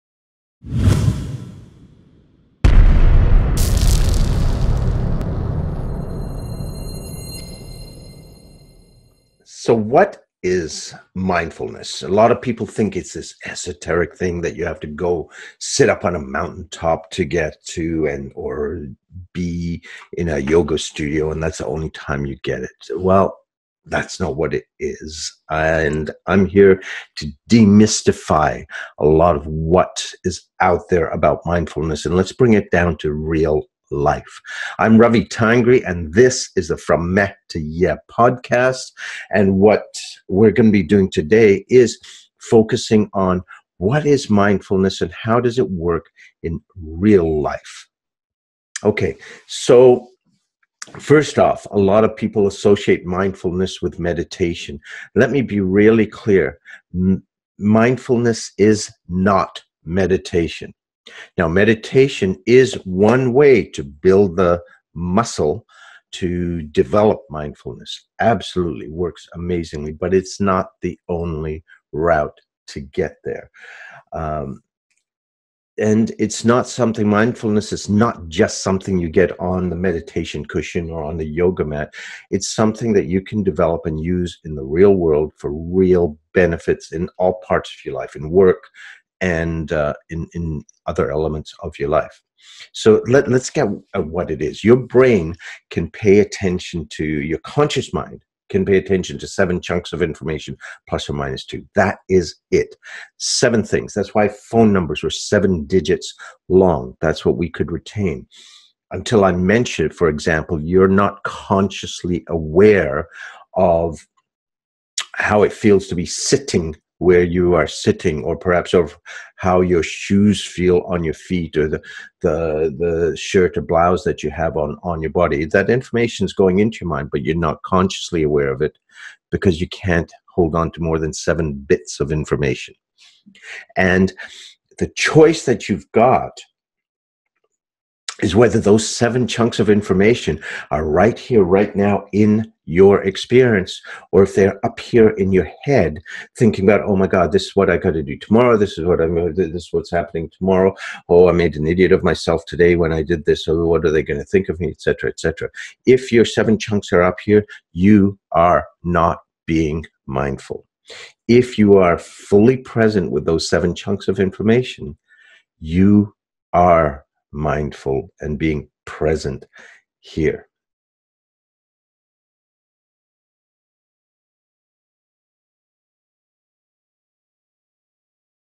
Demystifying Mindufulness meh2YEAH Pro File 180213
Mindfulness often seems to be some esoteric ability you only get if you sit and meditate on a mountaintop for years. Not true at all. In today's meh2YEAH! Pro-File, we demystify mindfulness and show what it really is and how to make it work in real life.